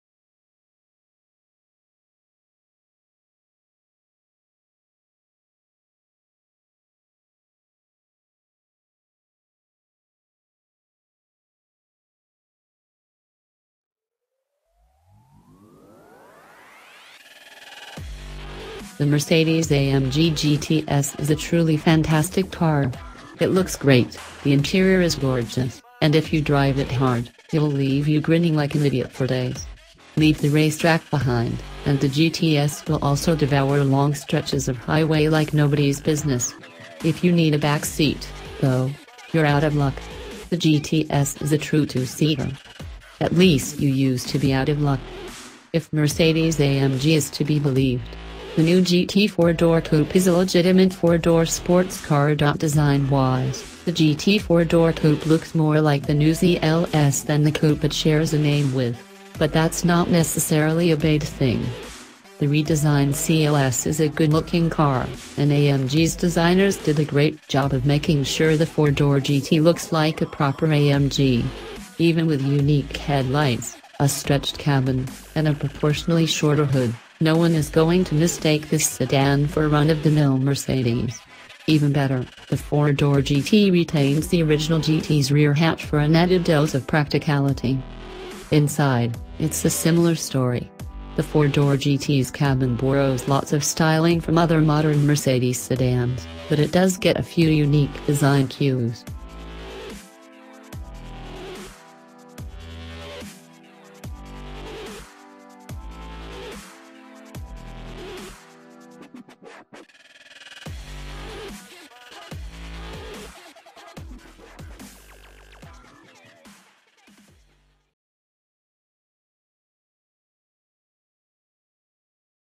Hot News !!! 2019 Mercedes AMG GT 63 S 4MATIC spec & price
Hot News !!! 2019 Mercedes AMG GT 63 S 4MATIC  spec & price - Just like the fast saloons that AMG has been making so well for decades, the new E 63 has a forced-induction V8 engine powerful enough to be worthy of a contemporary supercar. But unlike those forbears, it switches from one driven axle to two, and from coil springs to adaptive air suspension

██████ GET DAILY UPDATES! NOW! ██████